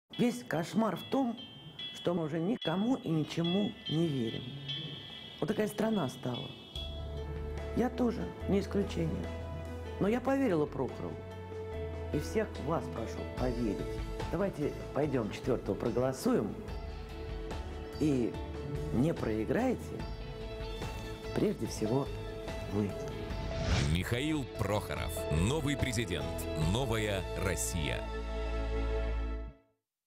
Предвыборный ТВ ролик. Алла Пугачева за Прохорова
Алла Пугачева.за Прохорова